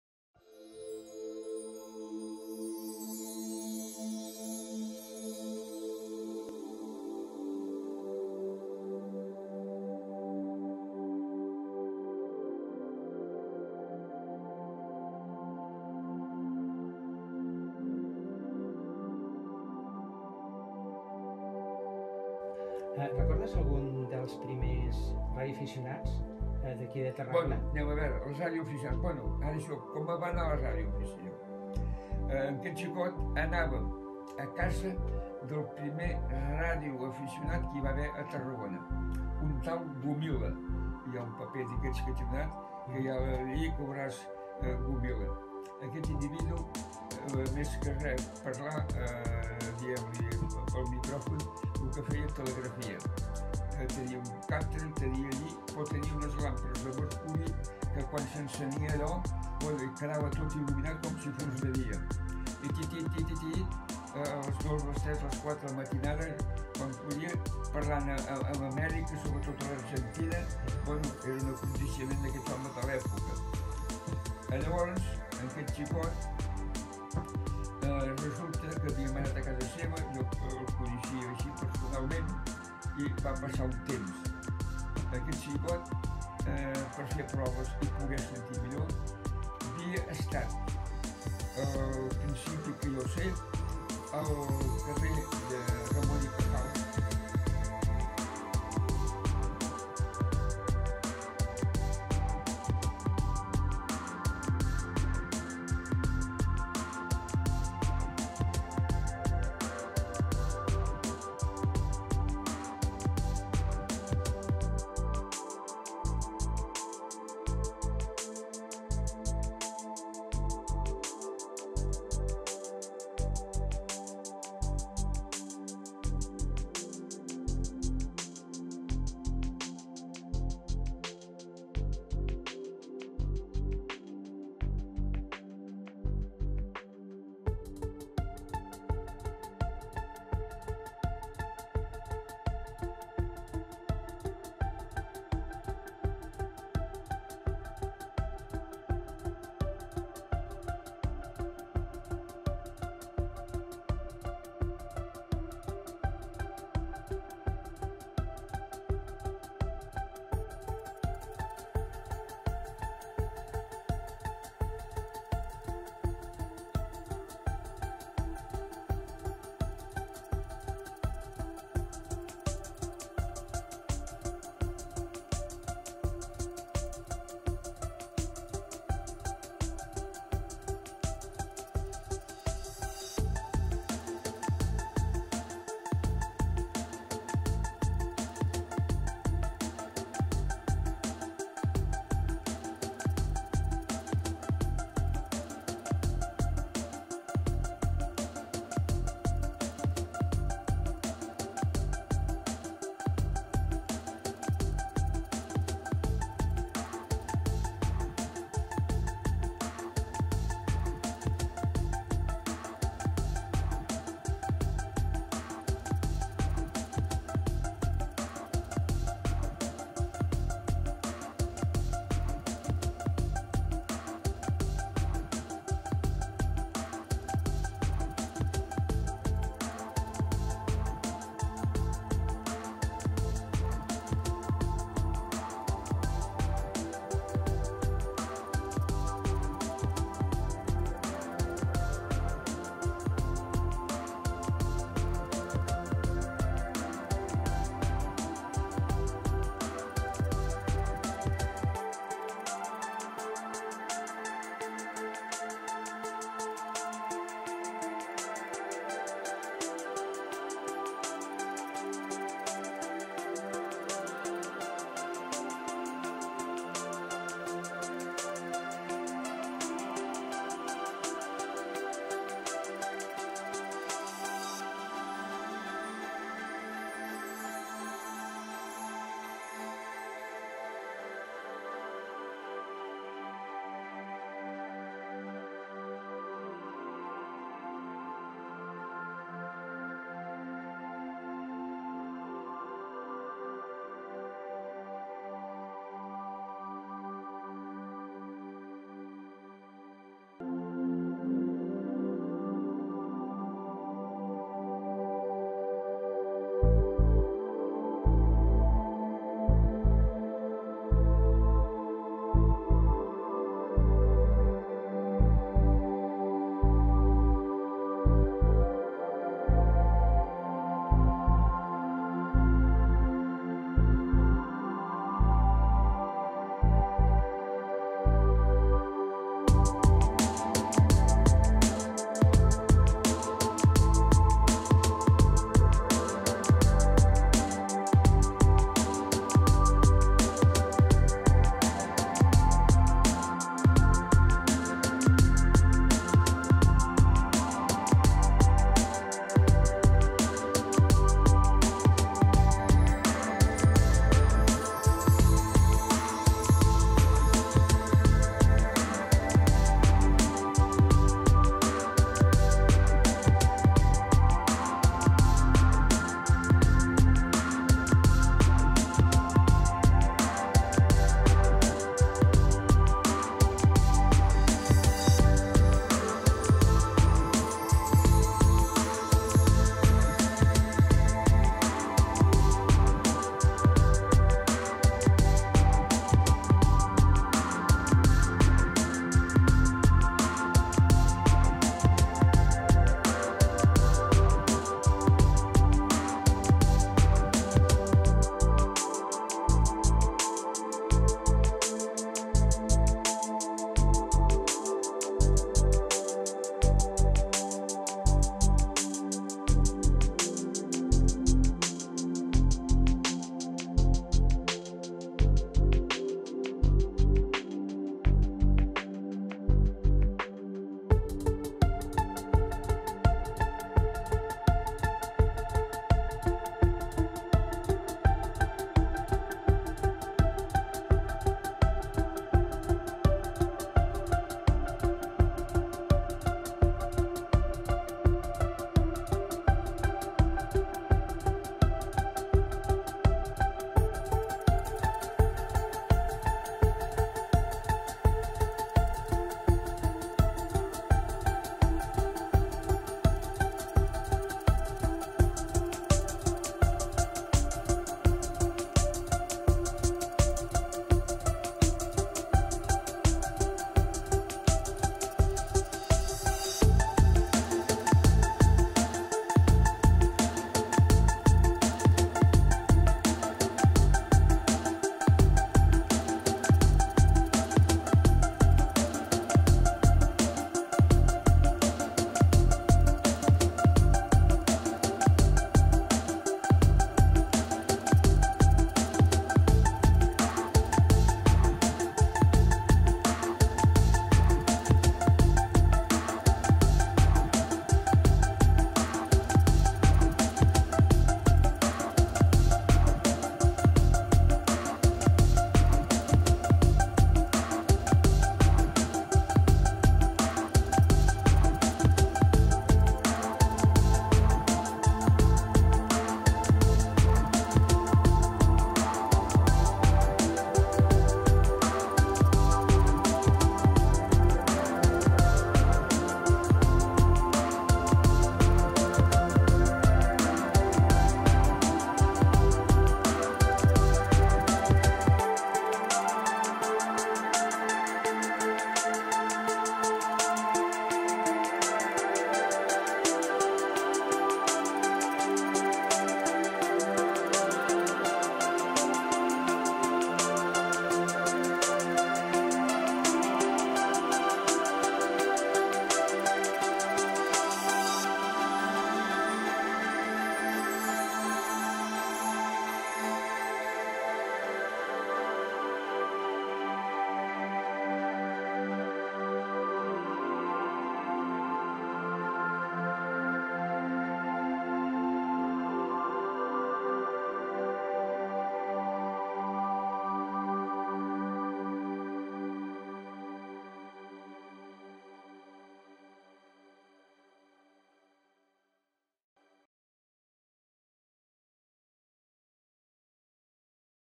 IberRadio 2017. Historia de Juan L. Gomila EAR-MT / EA3EG / EA6DD
Edicion para IberRadio. Juan Luis Gomila Mulet, el primer radioaficionado de Tarragona. Basado en un estudio de EA4DO, Isidoro Ruiz-Ramos,  editado por EA3HK, Pere